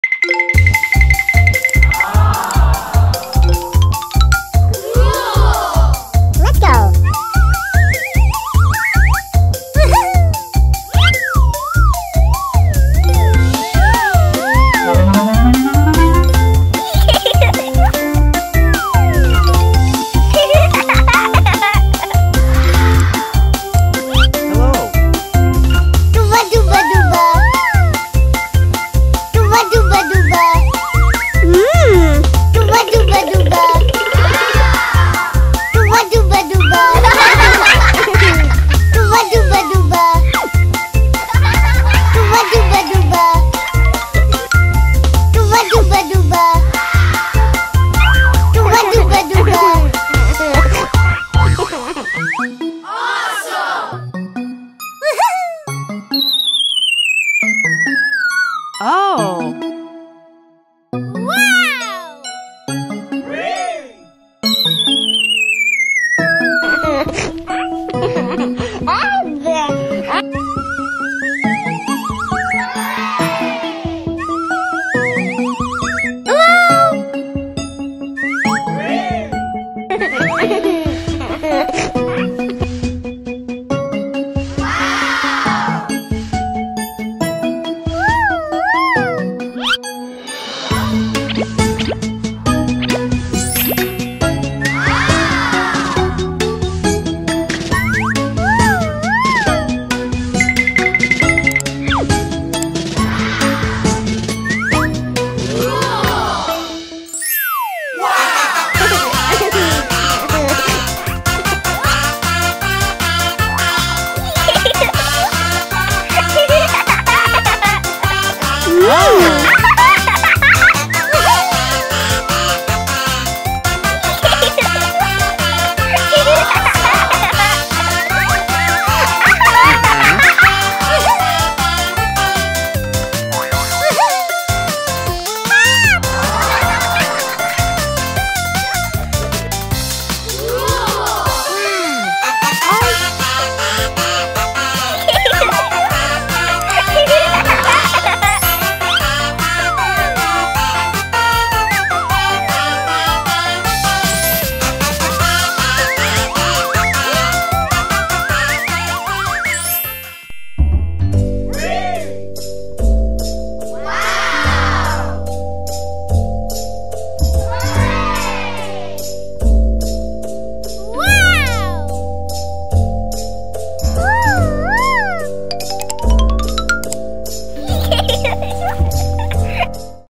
Margo plays at Indoor Playground For Kids
Games at indoor playground with balls pool. Learn colors and words with Margo at the play place.